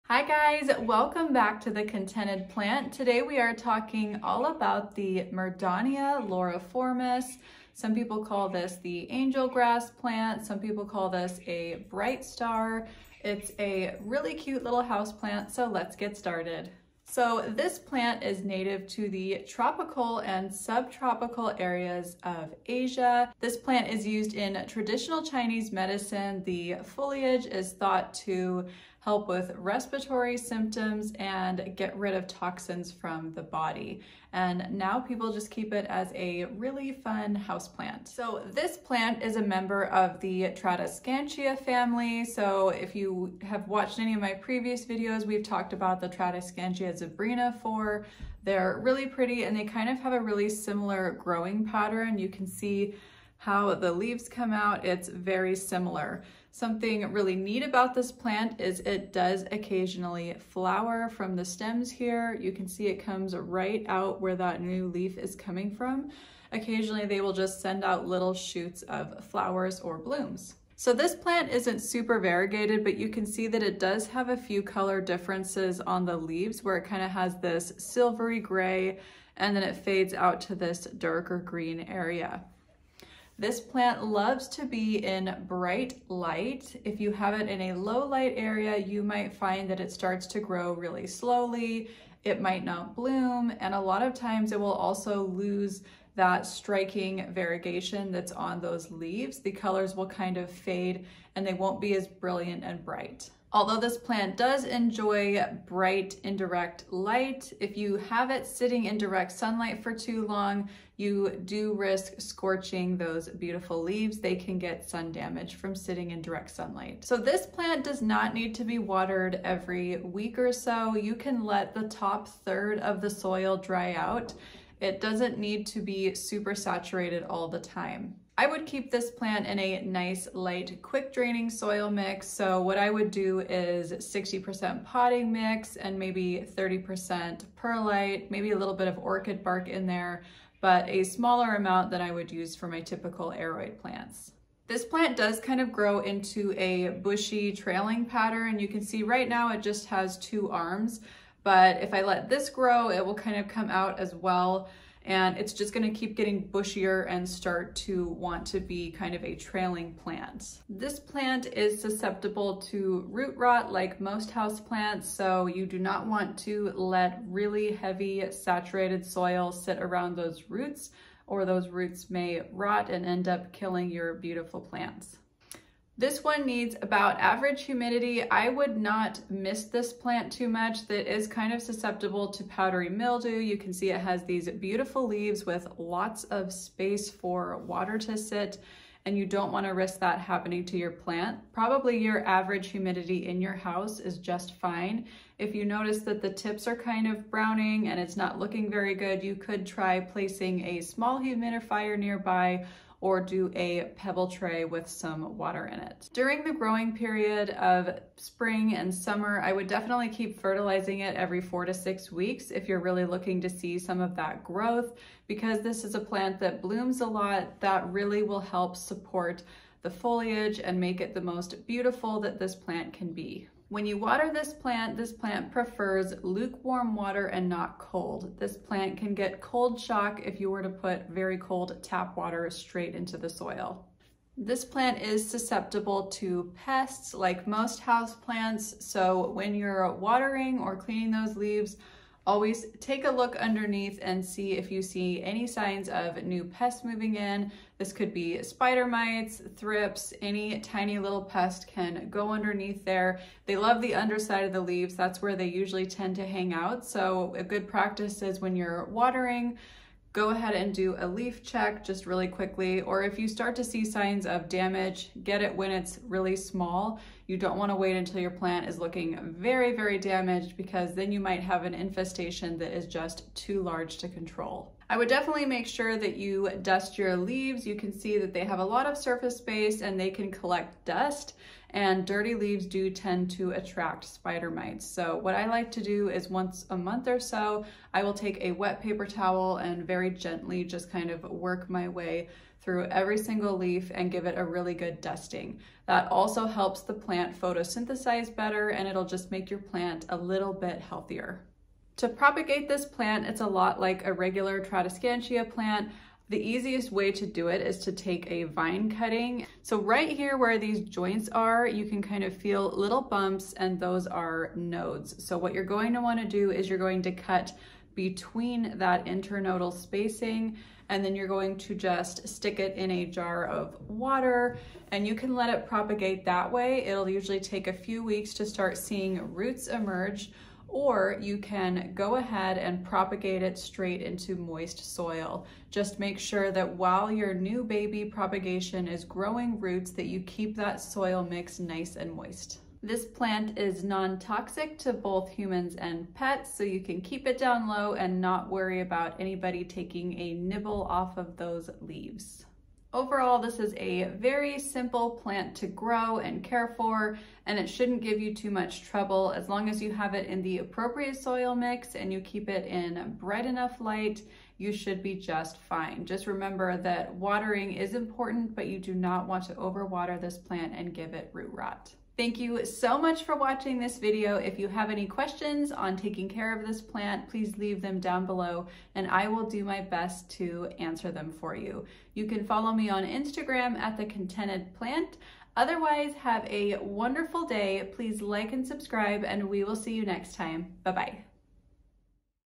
Murdannia Loriformis "Bright Star" Plant Care
Let's talk about the Murdannia Loriformis! This plant is pretty and easy to care for. I couldn't find a lot of information available for it, so I decided to make a video on it.

Plant Supply Links:

Clear Pots
Coco Coir Poles
Organic Perlite
Potting Mix
Orchid Bark